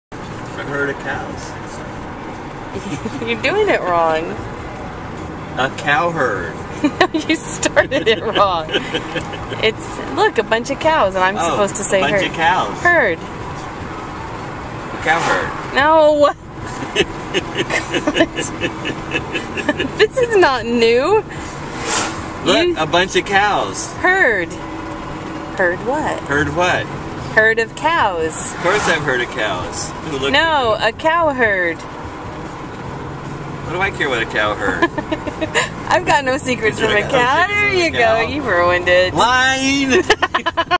Trying to get Mickey to tell a joke...
On the way home from running errands on a beautiful Saturday in February.